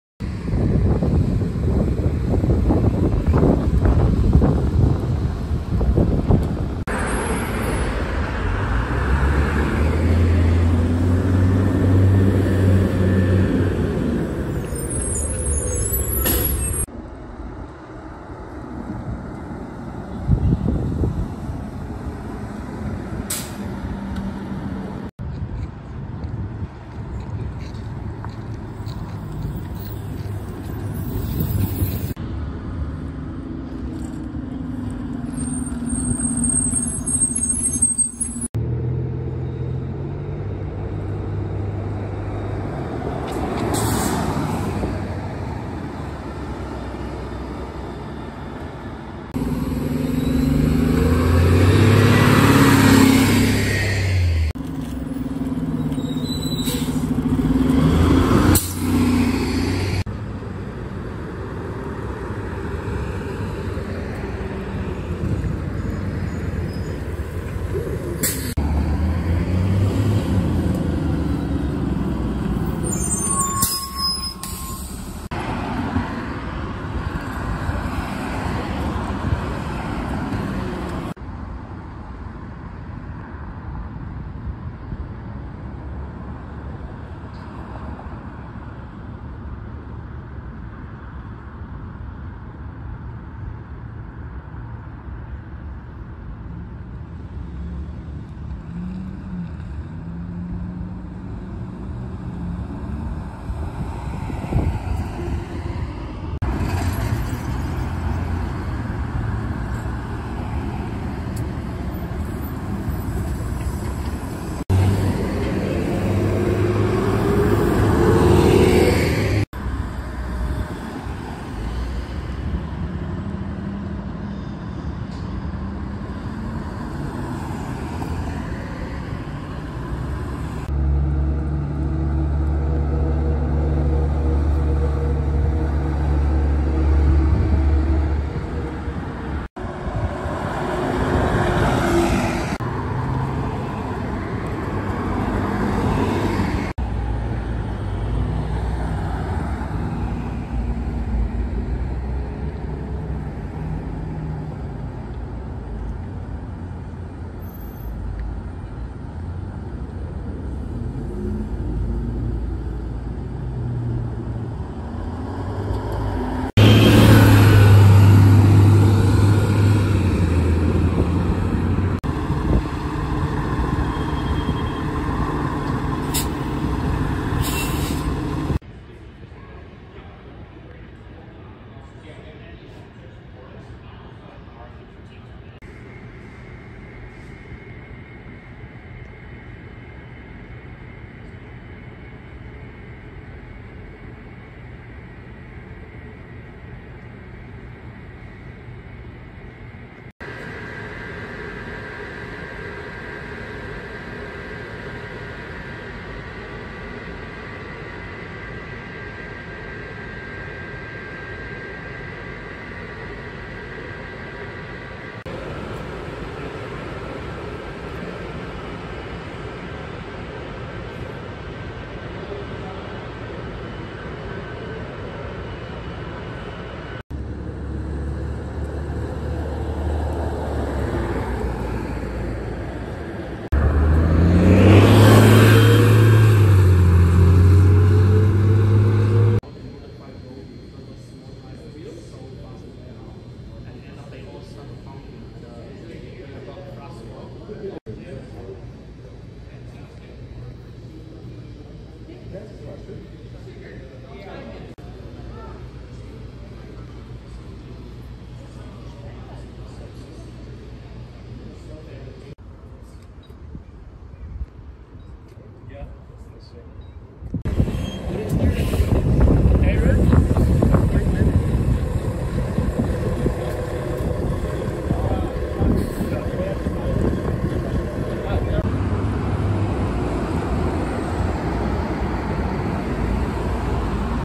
The Transit Show - Season 2 Episode 12
In this episode, transit vehicles are seen running around Edmonton, Alberta, Canada.

This episode also includes some exclusive shots inside two Edmonton Transit Service garages!